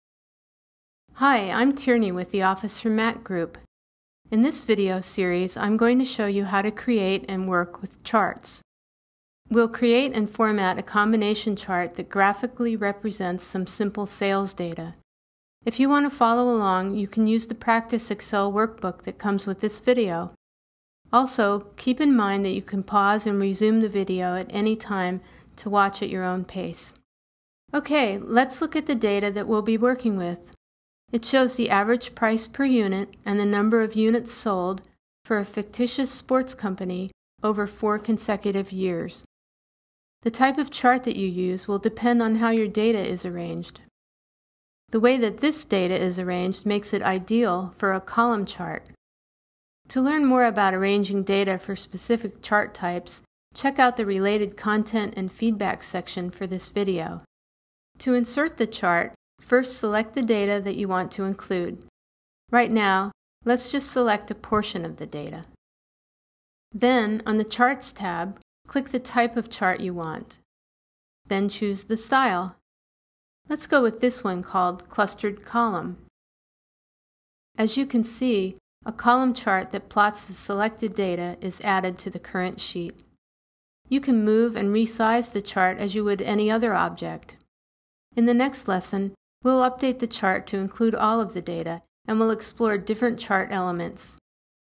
Lesson1 - Insert a chart in Excel 2011
Charts make numerical data visual. You can use charts to see at a glance what the data means and to share this information with others. Watch this series of videos to learn how.